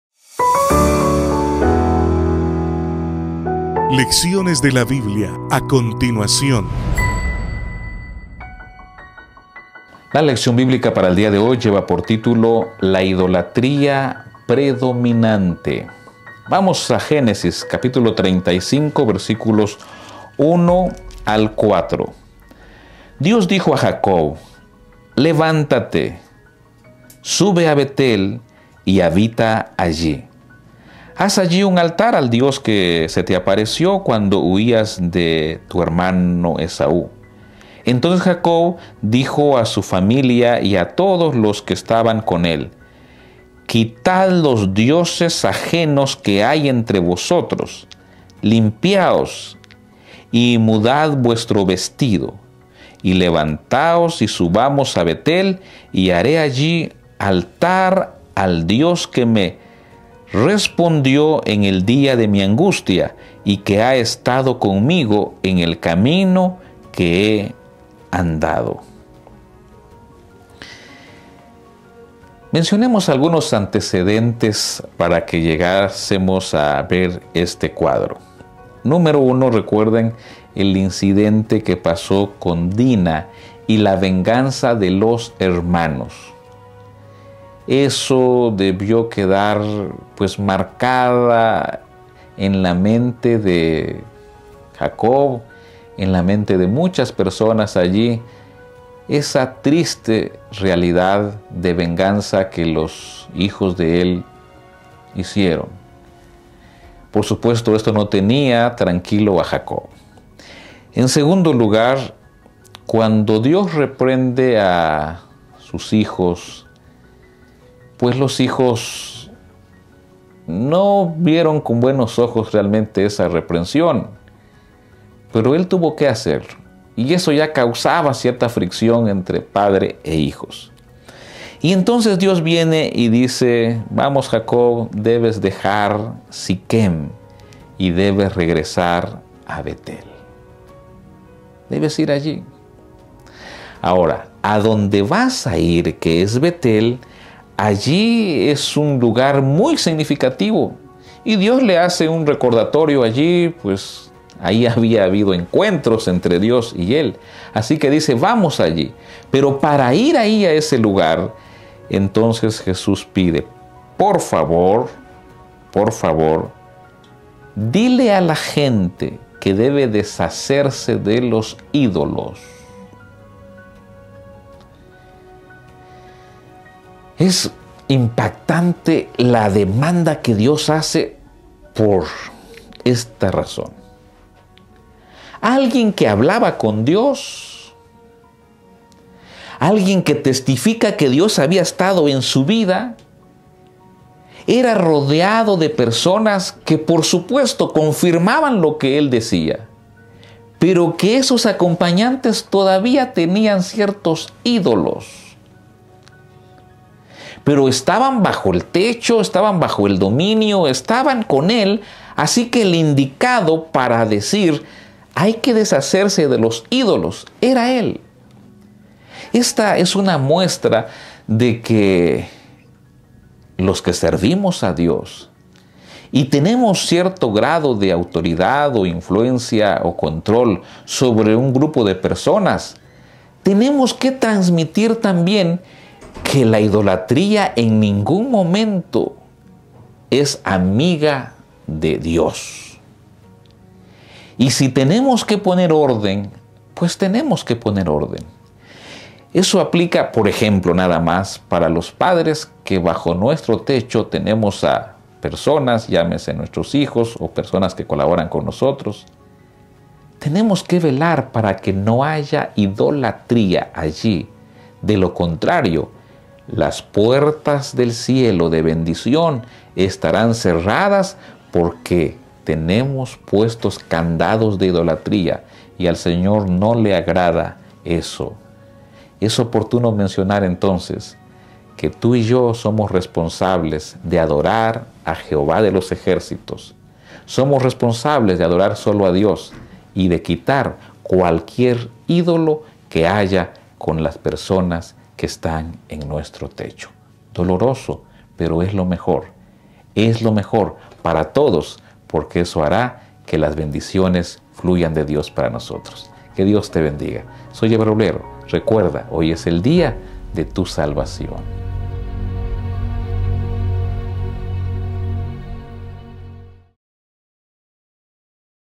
LDB - LA IDOLATRÍA PREDOMINANTE - 01 DE JUNIO 2022
La lección para este día se titula: LA IDOLATRÍA PREDOMINANTE, un tema presentado por el Pastor Ever Roblero.